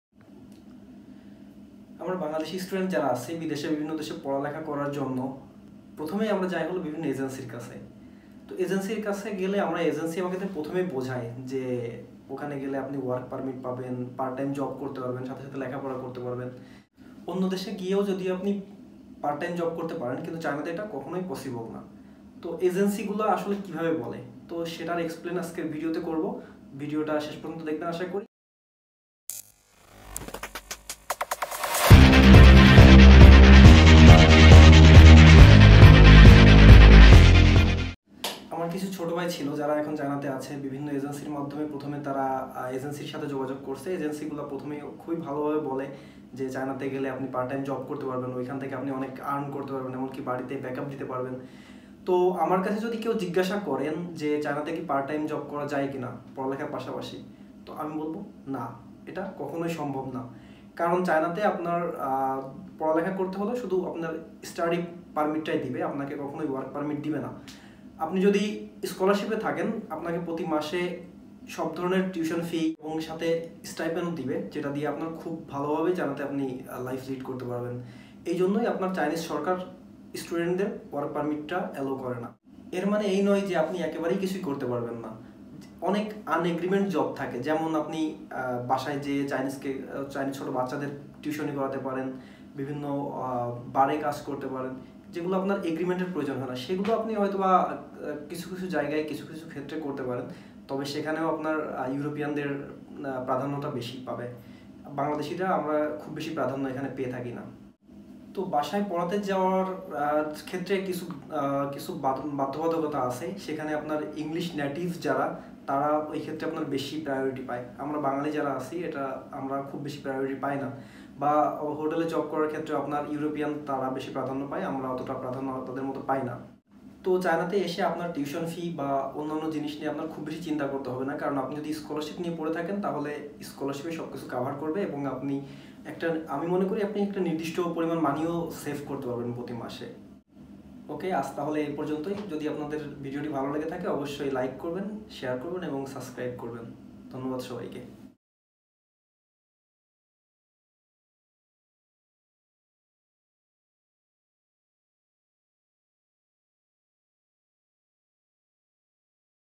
Can International Student work in china? Part time job in china for Bangladeshi student#studyinchina
বাংলাদেশি স্টুডেন্টরা কি চায়নাতে জব করতে পারে? সেখানকার বাস্তবতাটা কি!